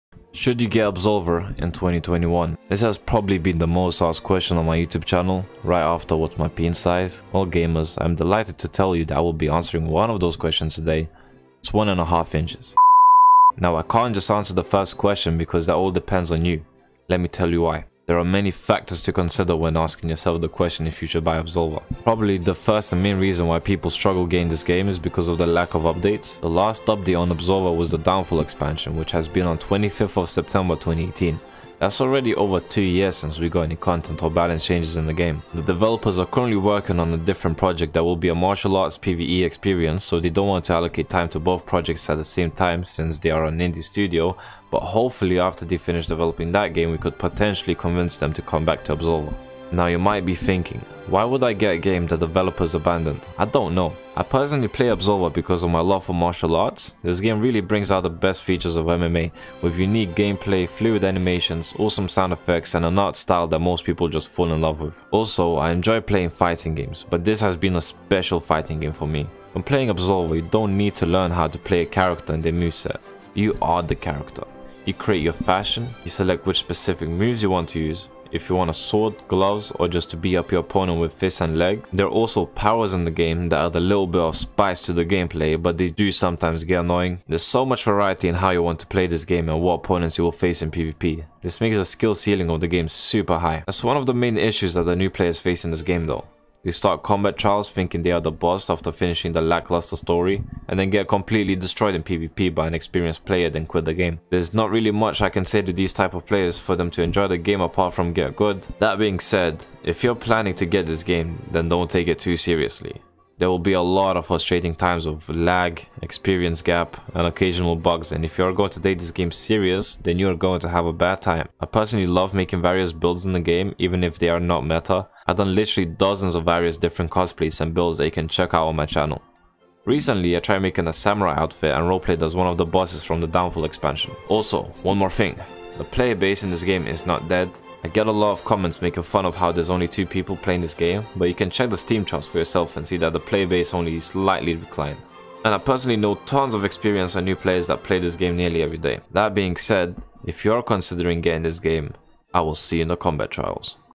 Should You Get Absolver In 2021?
Today I'm trying to answer the question that everyone keeps throwing at me, should you get Absolver? In this video I talk about the best and worst aspects of the game to help you make up your mind if this is the game for you. Personally, Absolver is still one of my favorite games and I enjoy it to this day. Even though the game doesn't get any updates anymore it is still a fun experience and if you have never tried it I would totally recommend it.

2021 has been a weird year for a lot of people, especially me. So when feeling down or if you just want to pass time you can always turn on this game to kick some ass either for 10 minutes or more, or get your ass kicked...

family friendly pg clean